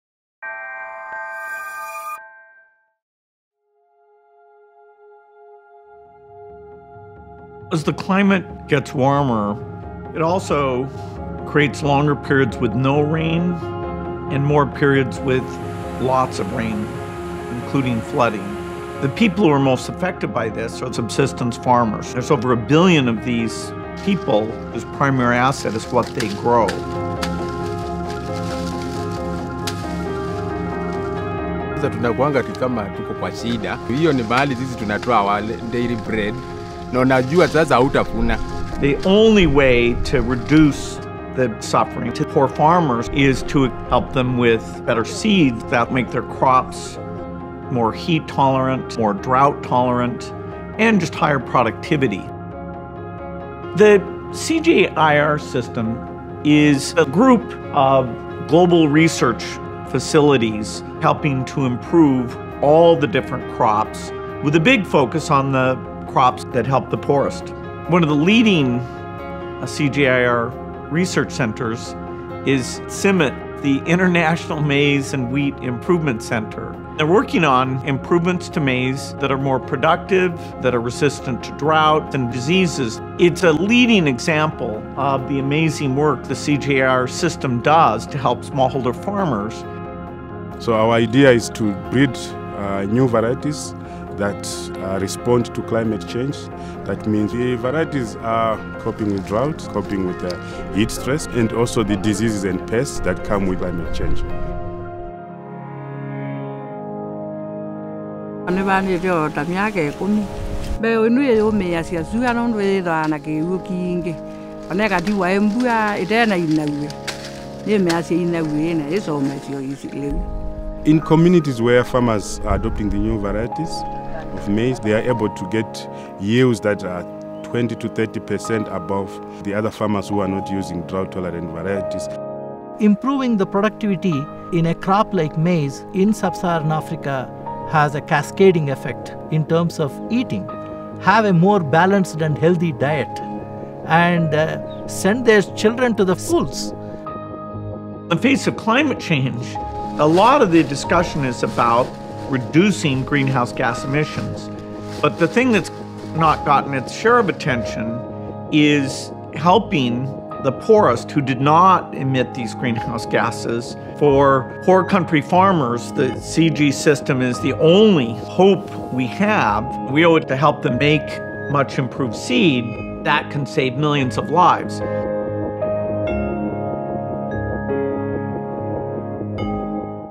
CGIAR: Feeding our future
You’ve probably never heard of CGIAR, but this little-known organization is essential to feeding our future in the fact of a growing population and climate change.